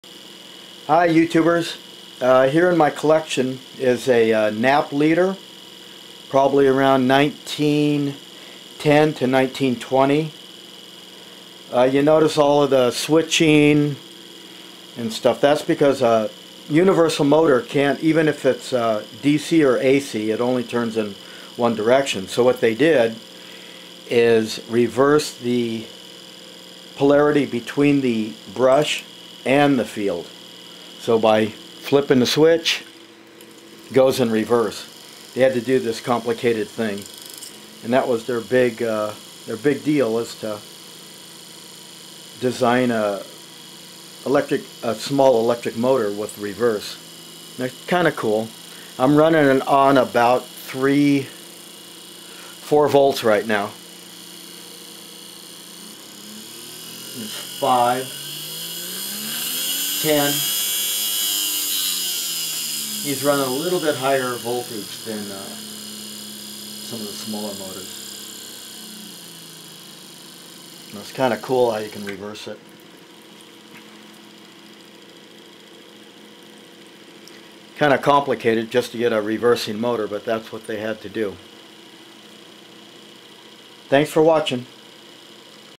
Antique Knapp Electrict Motor Reversible
This is a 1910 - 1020 Knapp Leader toy electric motor with reverse switch.  Universal motos  r only run in one direction AC or DC.  Knapp made this little switching box to change the polarity between the field and armature.